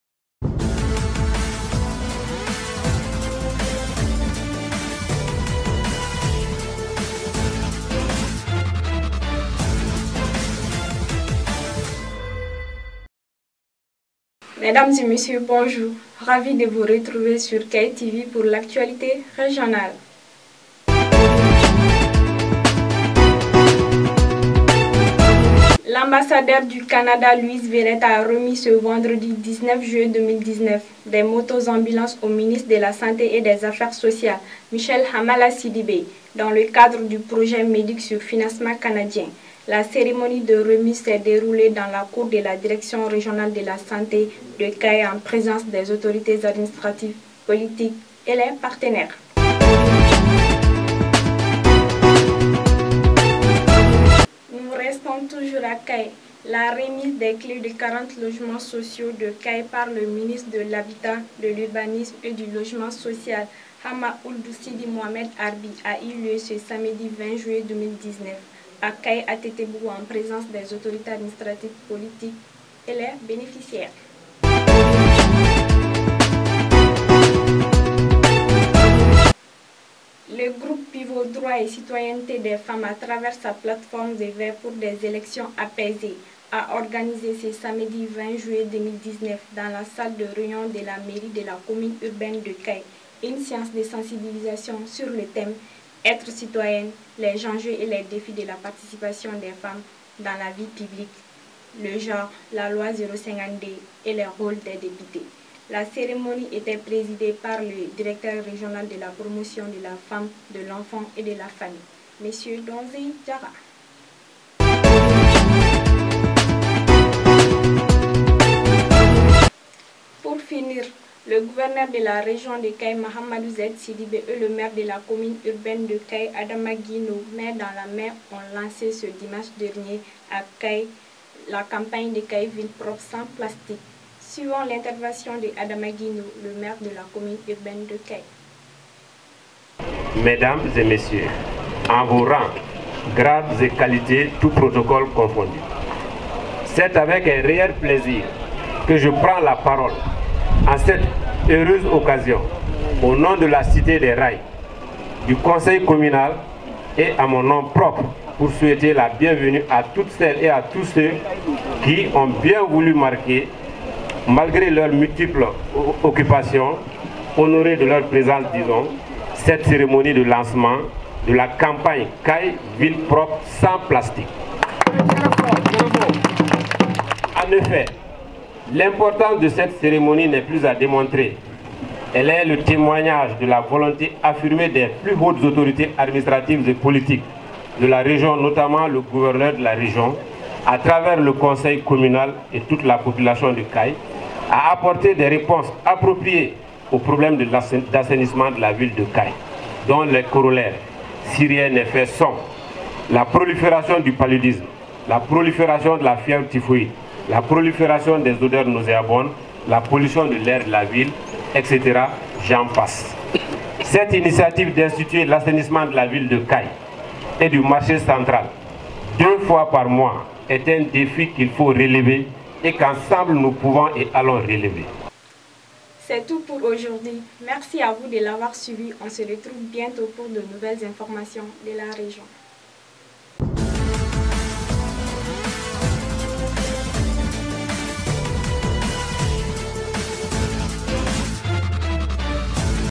Les dernières informations la région de Kayes sur la télévision web de Kayes
KAYESTV - Suivez Les dernières informations la région de Kayes sur la télévision web de Kayes avec la présentation de Mariam Tall.
Des sujets d'actualité de la région de Kayes ont été abordés notamment l'assainissement de la ville de Kayes.